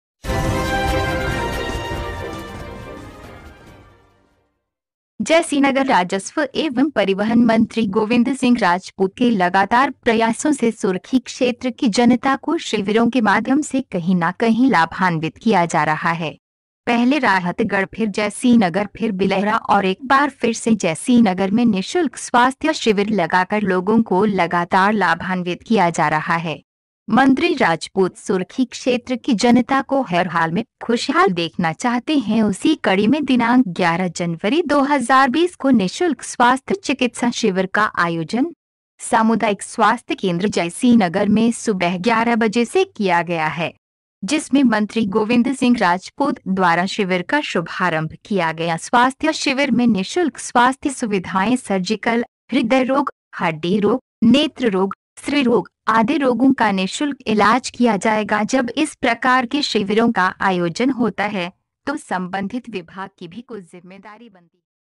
राजस्व एवं परिवहन मंत्री के अथक प्रयासों से जैसीनगर में फिर एक शिविर mpnewscast
राजस्व एवं परिवहन मंत्री के अथक प्रयासों से जैसीनगर में फिर एक शिविर

जैसीनगर :राजस्व एवं परिवहन मंत्री गोविंद सिंह राजपूत के लगातार प्रयासों से सुर्खी क्षेत्र की जनता को शिविरों के माध्यम से कहीं ना कहीं लाभान्वित किया जा रहा है पहले राहतगढ़ फिर जैसीनगर फिर बिलहरा और एक बार फिर से जैसीनगर में निशुल्क स्वास्थ्य शिविर लगाकर लोगों को लगातार लाभान्वित किया जा रहा है मंत्री राजपूत सुर्खी क्षेत्र की जनता को हर हाल में खुश हाल देखना चाहते हैं उसी कड़ी में दिनांक 11 जनवरी 2020 को निशुल्क स्वास्थ्य चिकित्सा शिविर का आयोजन सामुदायिक स्वास्थ्य केंद्र जेसीनगर में सुबह 11: 00 बजे से किया गया है जिसमें मंत्री गोविंद सिंह राजपूत द्वारा शिविर का शुभारंभ किया गया स्वास्थ्य शिविर में निशुल्क स्वास्थ्य सुविधाएं सर्जिकल, हृदय रोग ,हड्डी रोग ,नेत्र रोग, स्त्री रोग आदि रोगों का निशुल्क इलाज किया जाएगा जब इस प्रकार के शिविरों का आयोजन होता है तो संबंधित विभाग की भी कुछ जिम्मेदारी बनती है ल